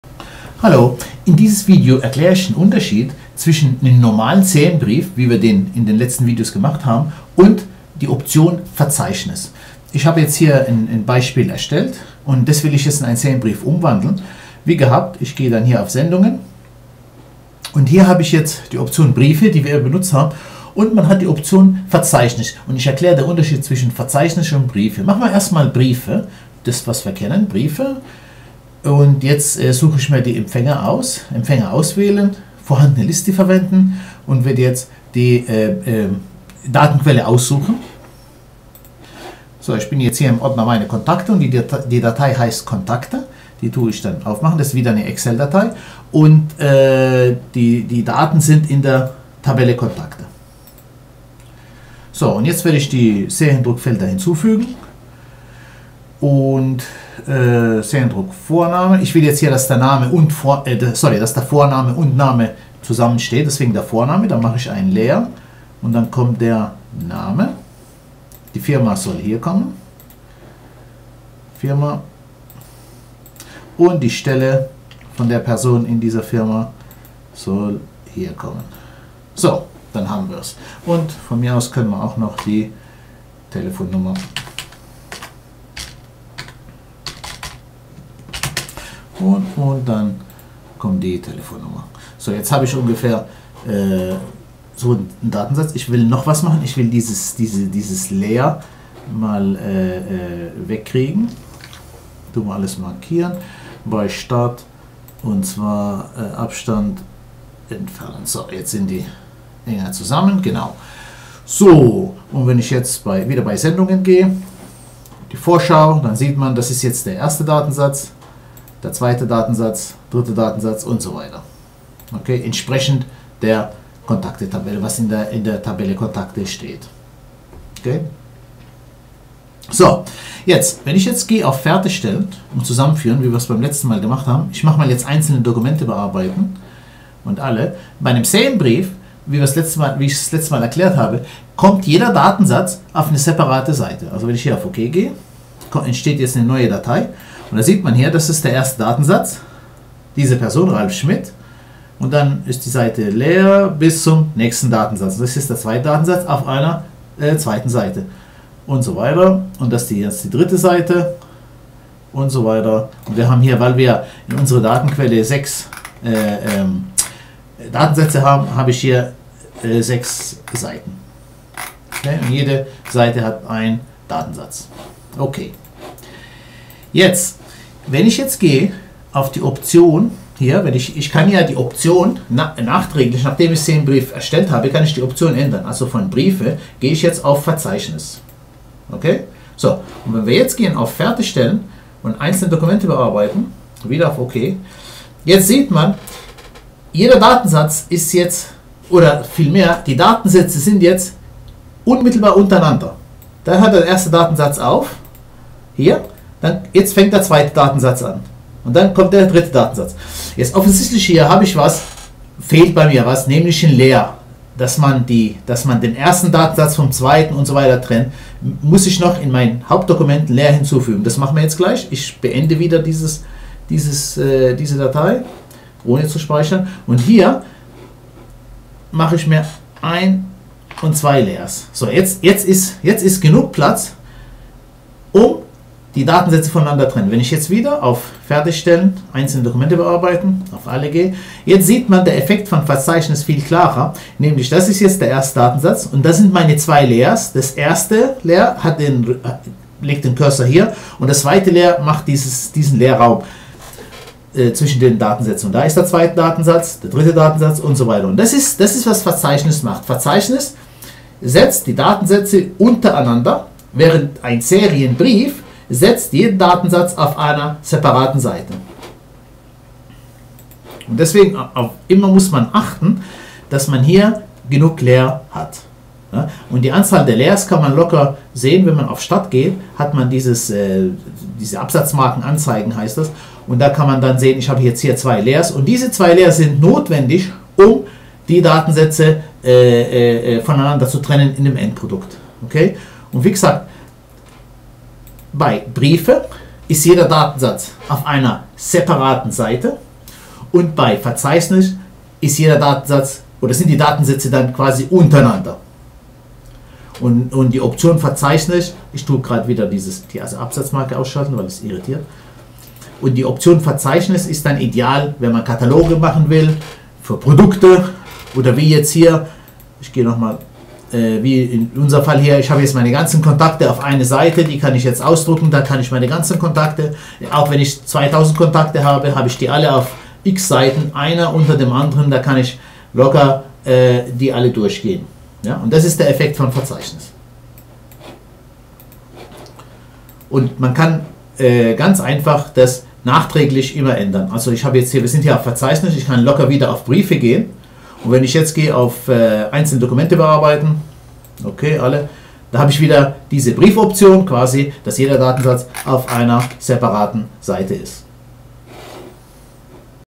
Serienbriefe mit Microsoft Word & Excel 4 – Unterschied zwischen der Option Briefe und Verzeichnis
Die Option Serienbriefe setzen im Endprodukt jeden Datensatz auf einer separaten Seite.

Bei der Option Verzeichnis dagegen stehen im Endprodukt die Datensätze untereinander.

Achten Sie darauf, daß Sie genug Leerzeilen am Ende des Textes haben, um die einzelnen Datensätze voneinander trennen.